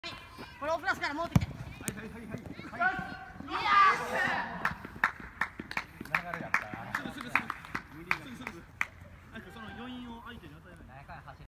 【ナイスアシスト】第4節20170312万博ロッソフットサル 長尾君→大塚 DFの間を切り裂くスルーパスからのゴール
トラップとシュートまで上手く持って行きました